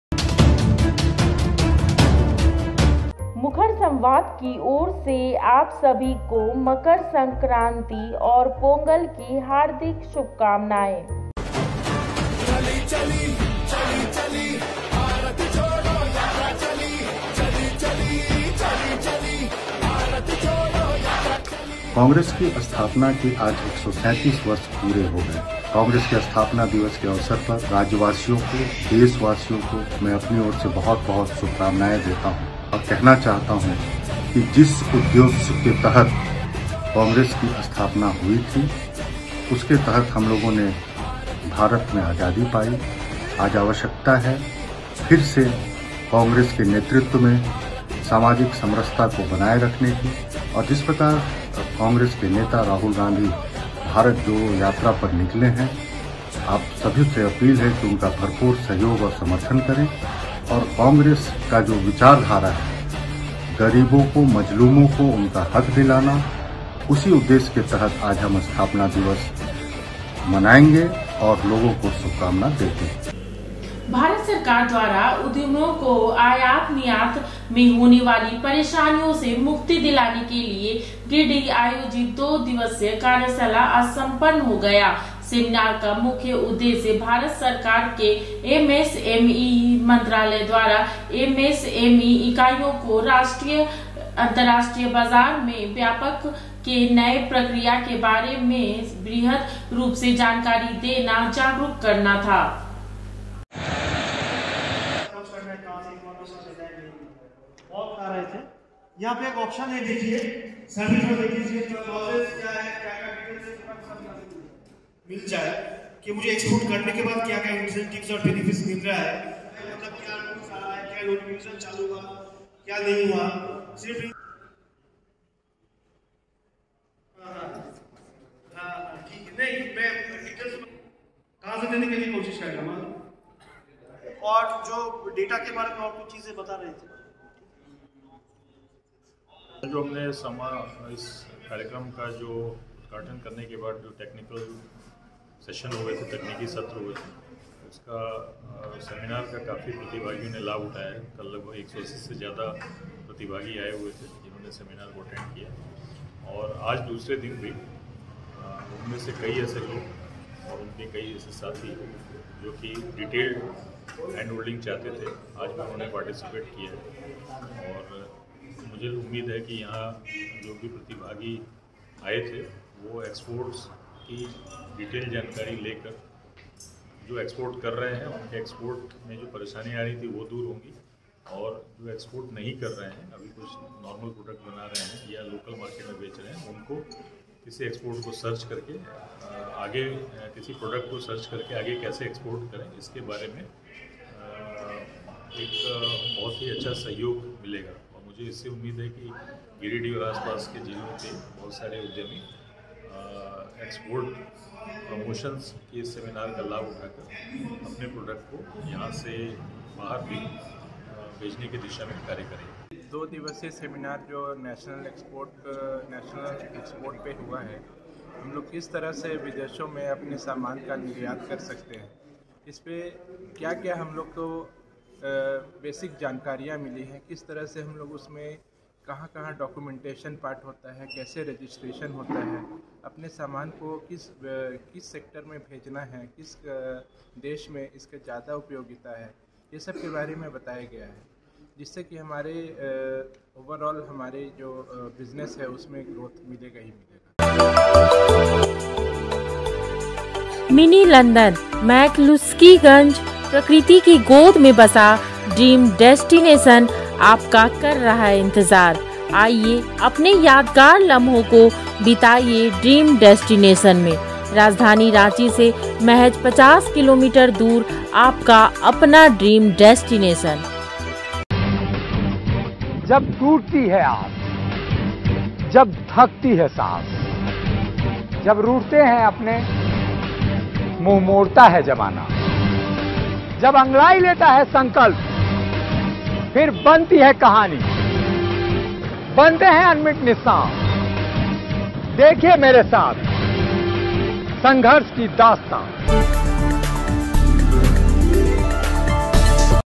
MSME उधमियों का दो दिवसीय कार्यशाला सम्पन्न,आयात-निर्यात पर दी गई सटीक जानकारी
भारत सरकार द्वारा उधमियों को आयात निर्यात   में होने वाली परेशानियों से मुक्ति दिलाने के लिए गिरिडीह आयोजित दो दिवसीय कार्यशाला सम्पन्न हो गया।

notification.